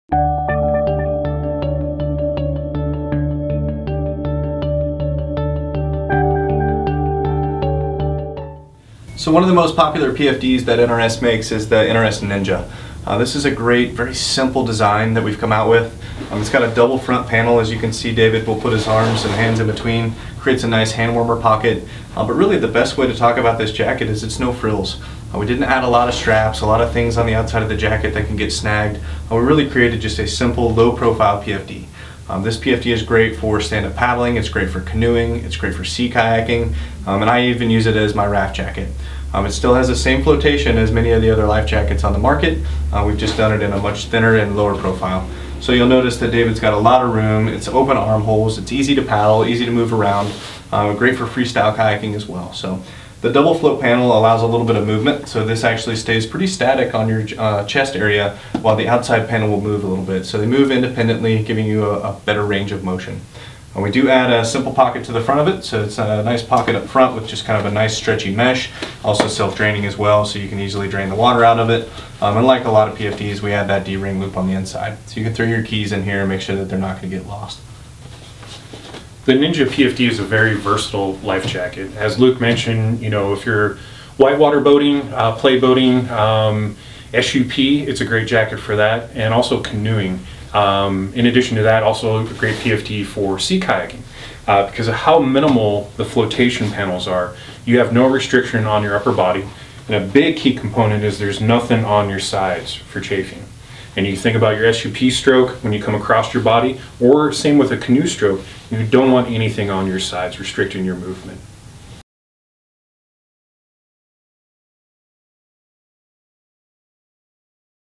NRS Ninja PFD
The NRS Ninja PFD Life Jacket - PFD is a low-profile life jacket. The NRS Ninja PFD is perfect for kayakers and boaters with short torsos. It's a compact design that won't interfere with paddling, rowing and swimming. A side entry, with two quick release buckles, is secure and easy to put on and take off. Check Fit Guide tab for proper sizing. Details